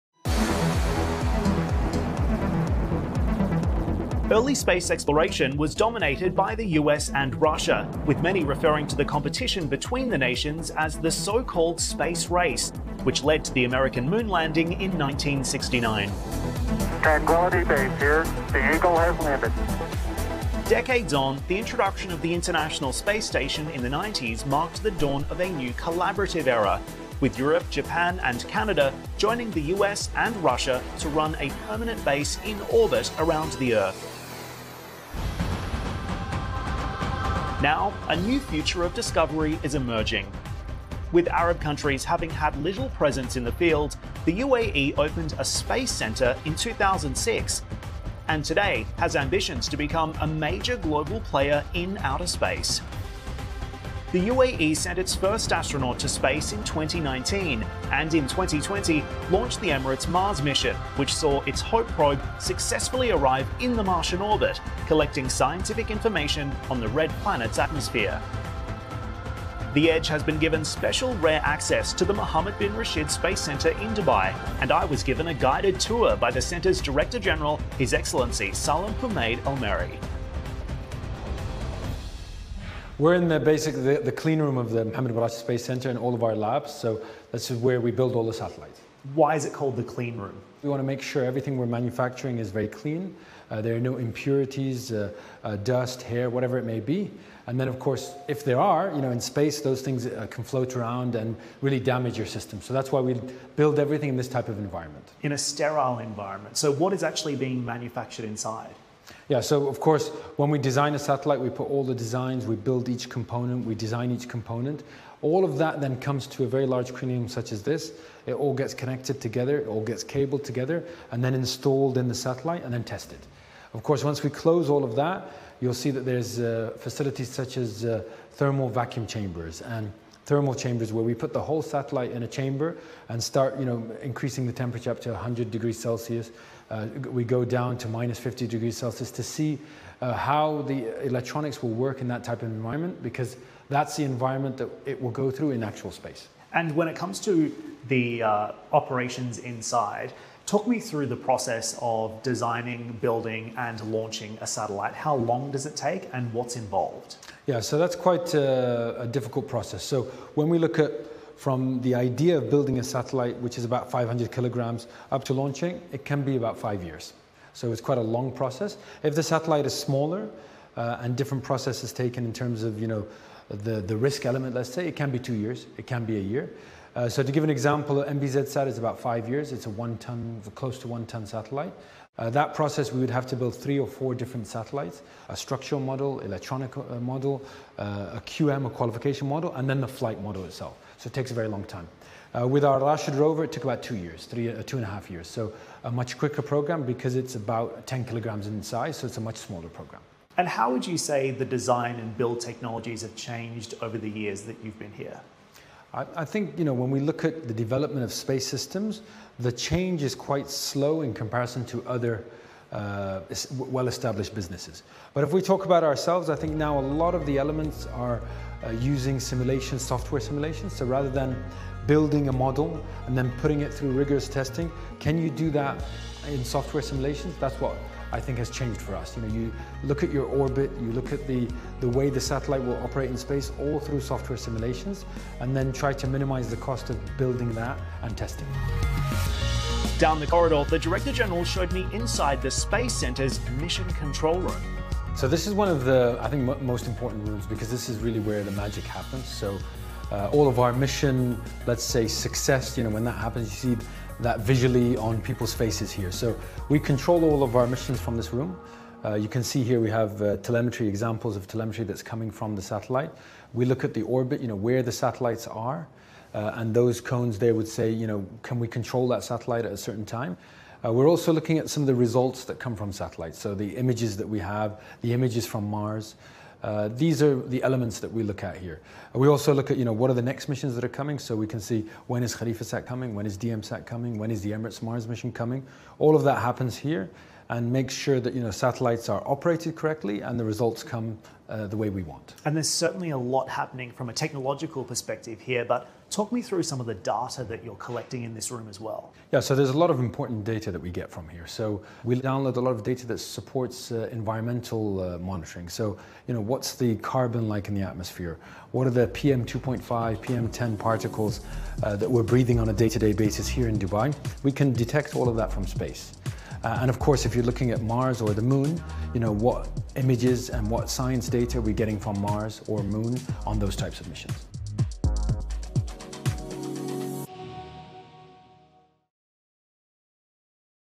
Inside the UAE's space centre
Salem Humaid Al Marri, the director general of the Mohammed Bin Rashid Space Centre in Dubai, gives CNBC's Dan Murphy a behind the scenes look at the satellite development facility – and inside the space center's mission control room.